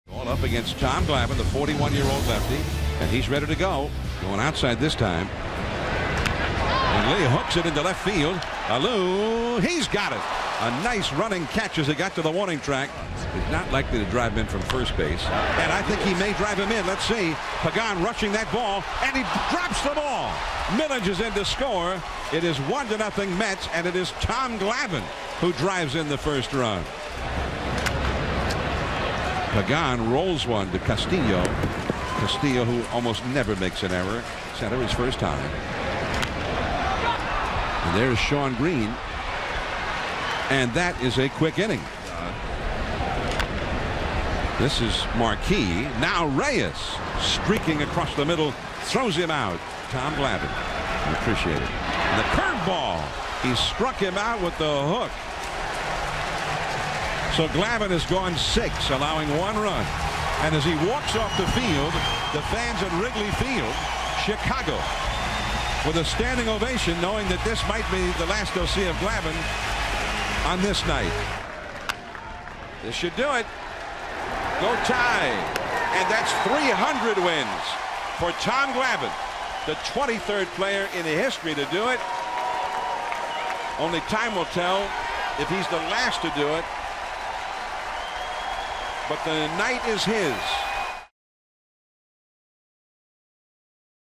NYM@CHC: Tom Glavine gets his 300th career win
8/5/07: Tom Glavine throws 6 1/3 innings, striking out one, giving up two unearned runs, and hitting an RBI single in his 300th win